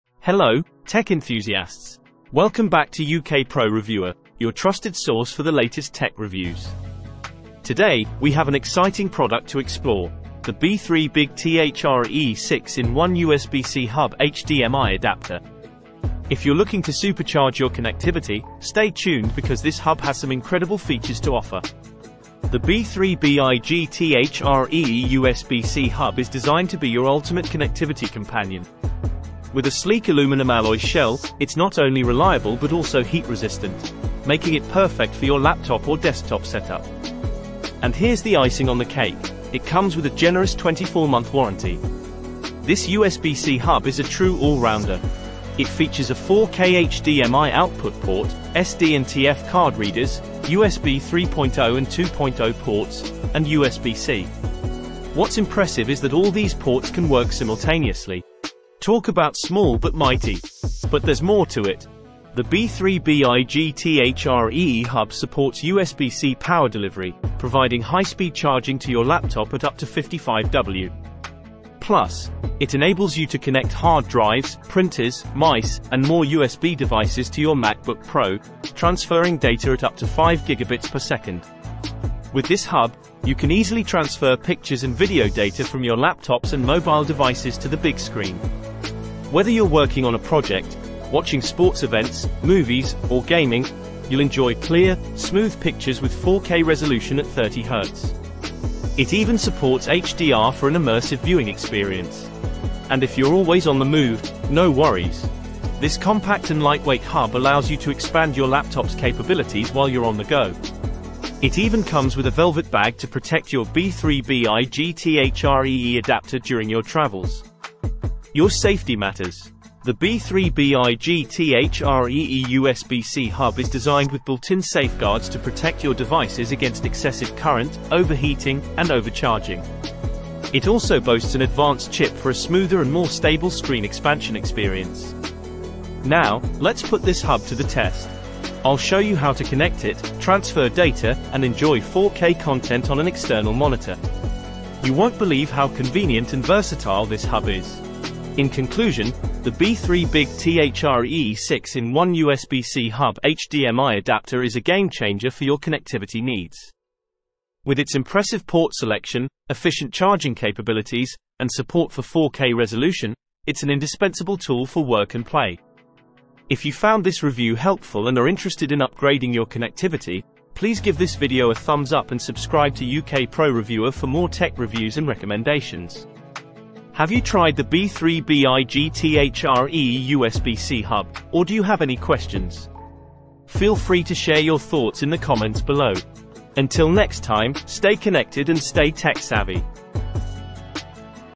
USB C Hub HDMI Adapter for MacBook Pro/Air Swith and Other Type C Devices Review
Amazon Product Name : USB C Hub HDMI Adapter for MacBook Pro/Air Swith and Other Type C Devices

Welcome to our USB C Hub HDMI Adapter Review! 🔌 In this video, we explore the versatility and functionality of this handy USB C hub designed for MacBook Pro/Air and other Type C devices. Discover how this adapter can expand your connectivity options, including HDMI output, and make your daily tasks more efficient. Whether you're a MacBook user or have other Type C devices, this review will provide valuable insights into the performance and convenience of this adapter. 🖥️✨